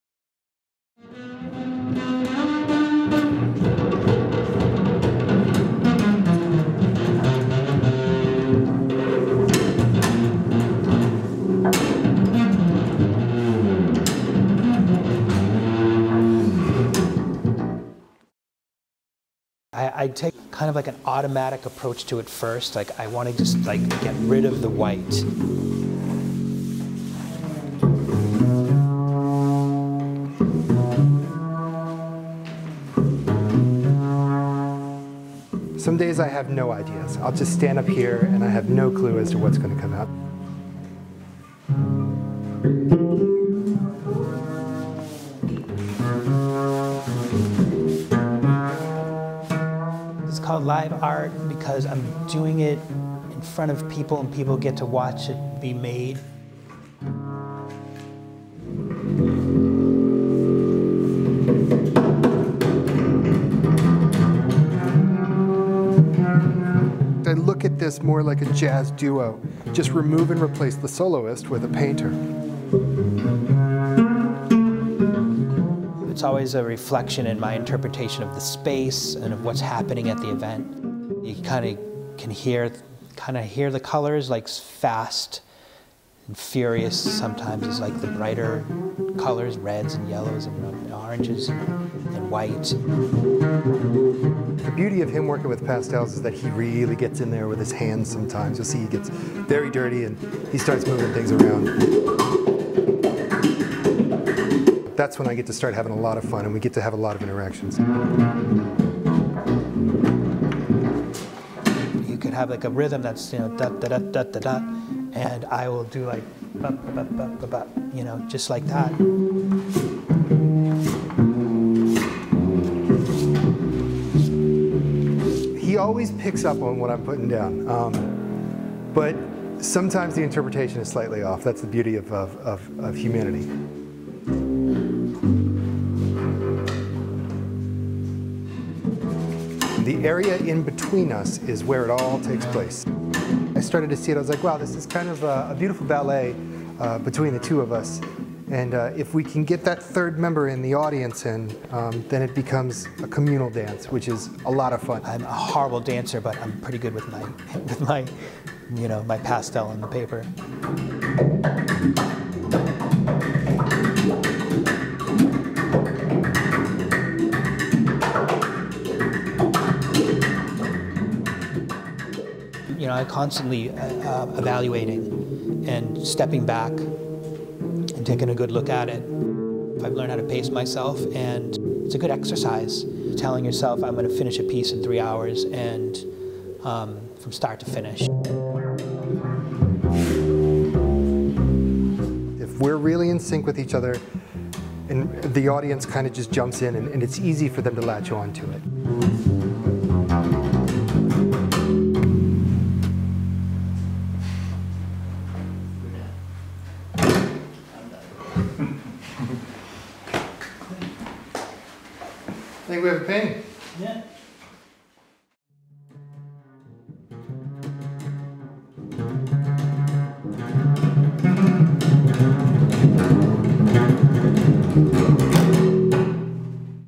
A Live Art Performance by Tabula Rasa
Tabula Rasa is Jeffrey Lipsky and Bryan Worley.  Lipsky creates a pastel masterpiece in 2-3 hours accompanied by the stand-up bass improvisational stylings of Bryan Worley.  They play off each other and the music creates the art creates the music.
Produced by Lowellia Films